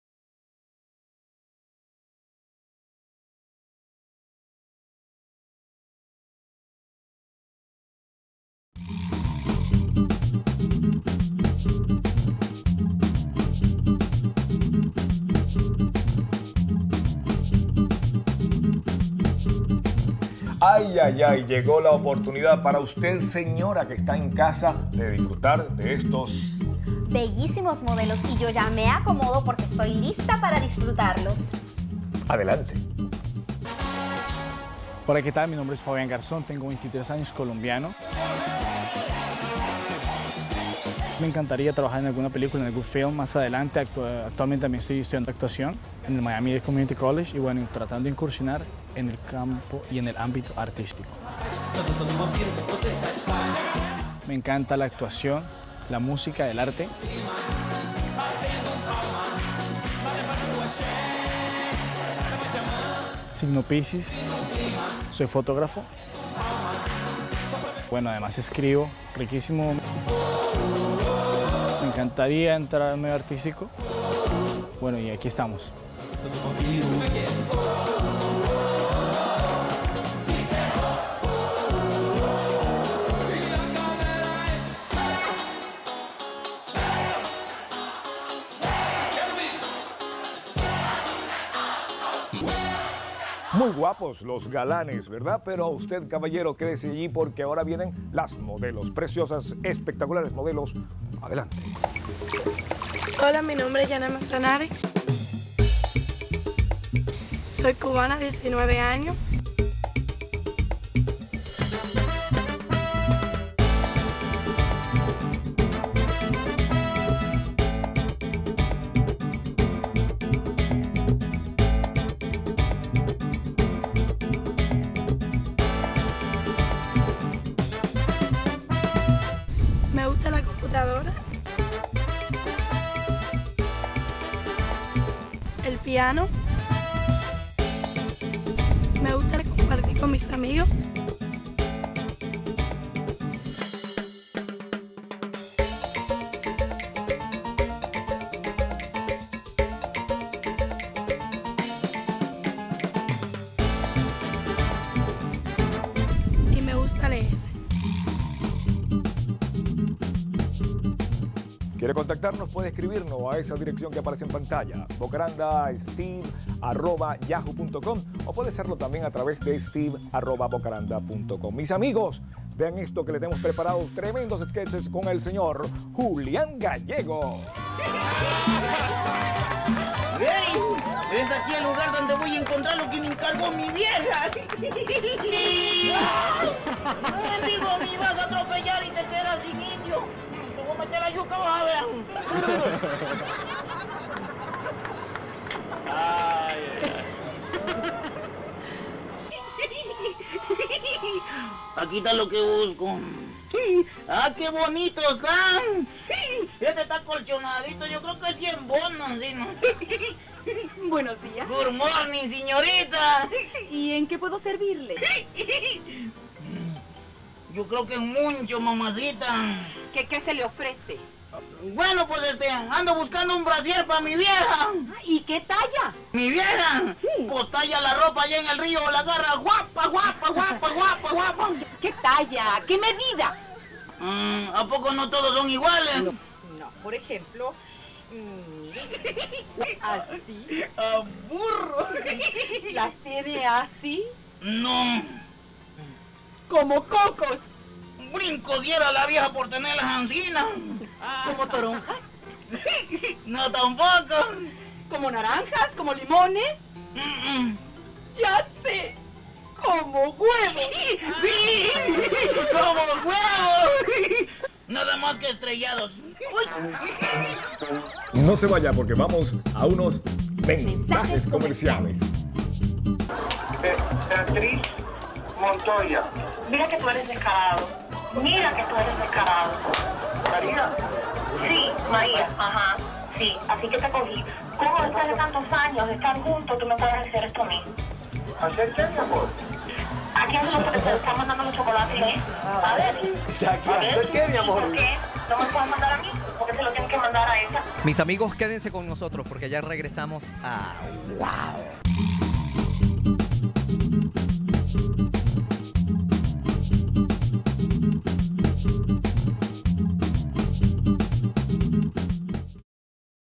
Los Modelos y actores En Miami
Este es el programa Wow, el cual tenemos invitados especiales modelos de Colombia y Cuba con la animacion de Steve Bocaranda

Actores y modelos de Venezuela, Colombia, México, Perú, Bolivia, USA, España, Valencia, Argentina, Panama, Guatemala, El Salvador, Honduras, etc .

Bocaranda Show Productions, Inc.
Director y Editor: Steve Bocaranda